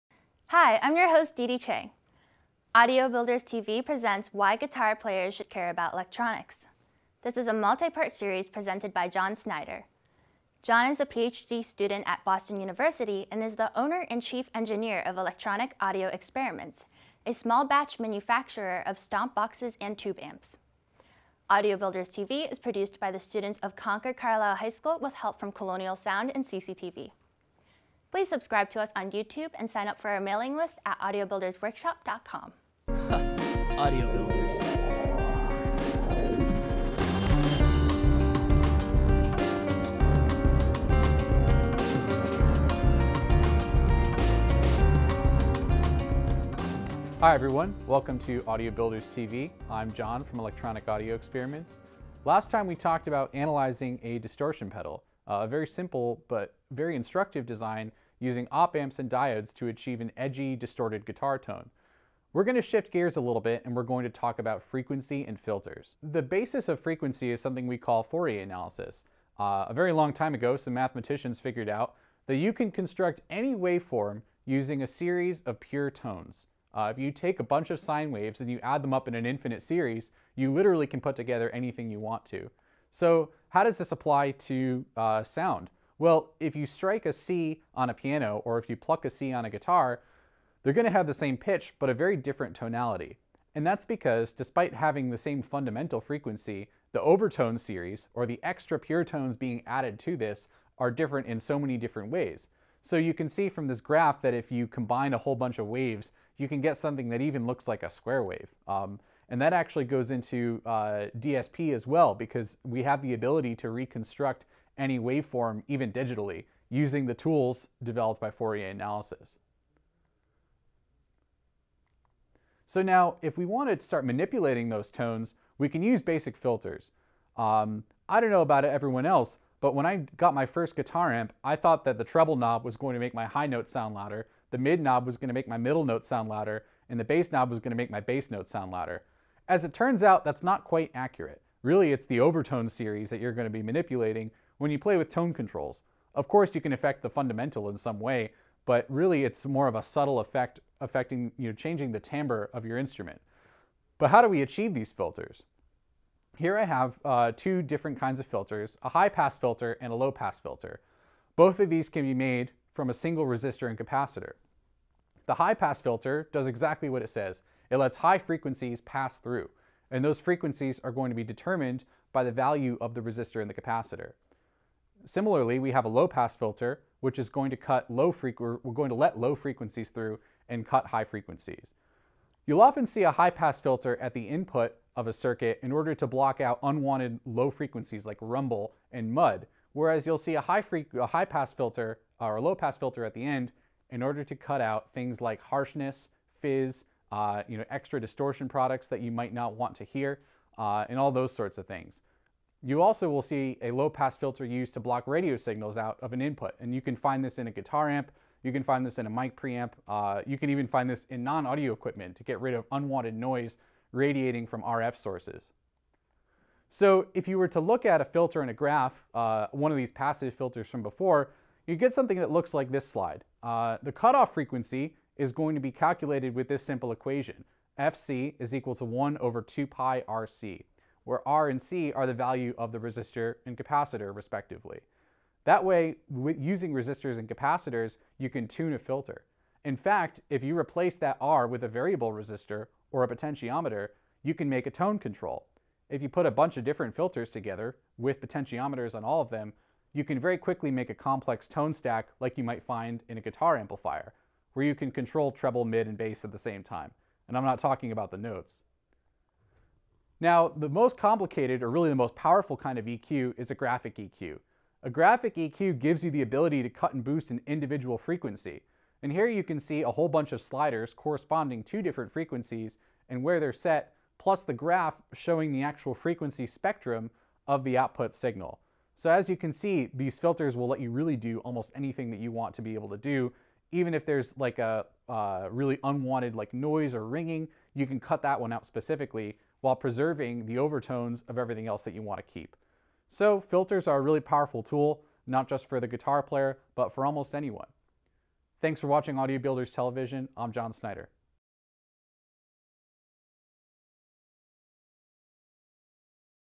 Guitar Players Care About Electronics Part 5 - John Snyder
Audio Builders TV presents “Why Guitar Players Should Care About Electronics.” This is a multi-part series presented by John Snyder. John is a PhD student at Boston University and is the owner and chief engineer of Electronic Audio Experiments, a small batch manufacturer of stompboxes and tube amps . Audio Builders TV is produced by the students of Concord Carlisle High School with help from Colonial Sound and CCTV.